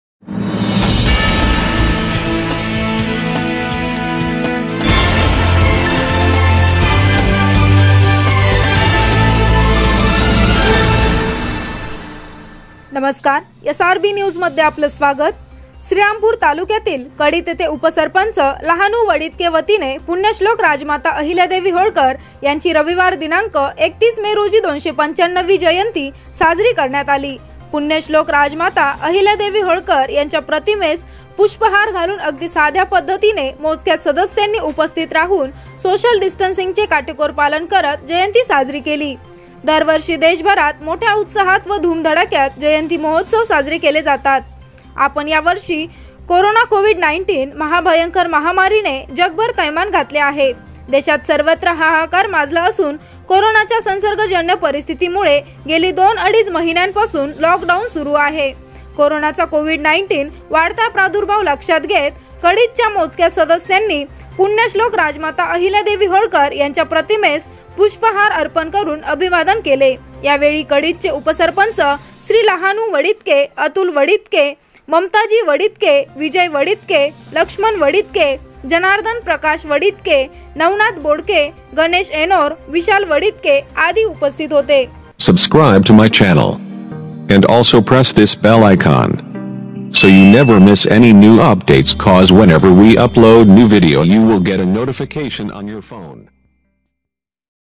कडीत येथे पुण्यश्लोक राजमाता अहिल्यादेवी होळकर यांची 295 वी जयंती साजरी
श्रीरामपूर तालुक्यातील कडीत येथे उपसरपंच लाहनु वडीतके वतीने  पुण्यश्लोक राजमाता अहिल्यादेवी होळकर यांची आज रविवार दि. 31 मे रोजी 295 वी जयंती साजरी करण्यात आली पुण्यश्लोक राजमाता अहिल्यादेवी होळकर यांच्या प्रतिमेस पुष्पहार घालून अगदी साध्या पद्धतीने मोजक्याच सदस्यांनी उपस्थित राहून सोशल डिस्टन्सिंगचे काटेकोर पालन करत जयंती साजरी केली.

दरवर्षी देशभरात मोठ्या उत्साहात व धुमधडाक्यात जयंती महोत्सव साजरे केले जातात. पण यावर्षी कोरोना (कोविड 19) महाभयंकर महामारीने जगभर थैमान घातले आहे. देशात सर्वञ हाहाकार माजला असून कोरोनाच्या संसर्गजन्य परीस्थितीमुळे गेली दोन अडिच महिन्यापासून लॉकडाऊन सुरु आहे. कोरोनाचा (कोविड 19) वाढता प्रादुर्भाव लक्षात घेत कडीतच्या मोजक्याच सदस्यांनी पुण्यश्लोक राजमाता अहिल्यादेवी होळकर यांच्या प्रतिमेस पुष्पहार अर्पण करून अभिवादन केले. यावेळी कडीतचे उपसरपंच श्री लाहुनु वडीतके अतुल वडीतके ममताजी वडीतके विजय वडीतके लक्ष्मण वडीतके जनार्दन प्रकाश वडीतके नवनाथ बोडके गणेश ऐनोर विशाल वडीतके आदी उपस्थित होतेSRBNEWS#